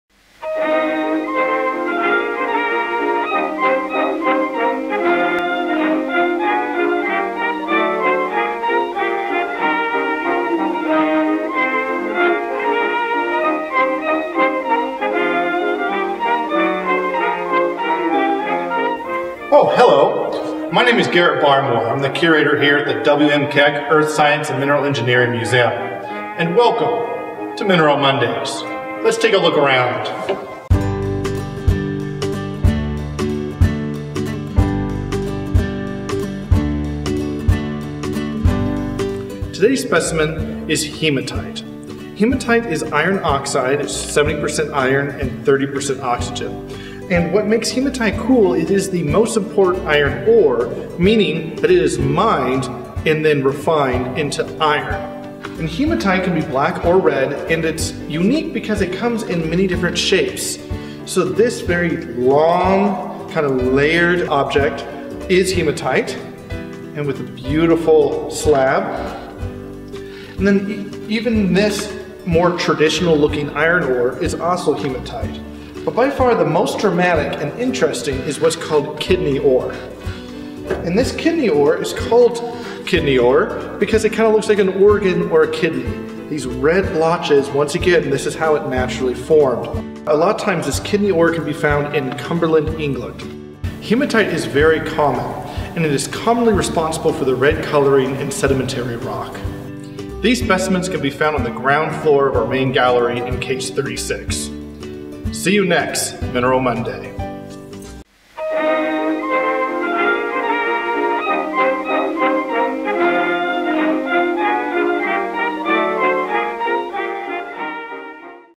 Mineral Monday: Hematite
Each Monday, join museum curator Garrett Barmore on an educational exploration of the many amazing minerals, fossils and historical objects on display at the W.M. Keck Earth Science and Mineral Engineering Museum! This week, Garret brings out four different formations of hematite.